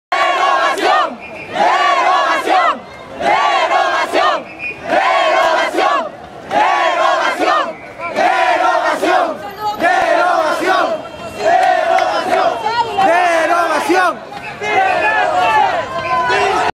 Marcha: No al régimen laboral juvenil
Los jóvenes somos pacíficos, tus hijos están aquí!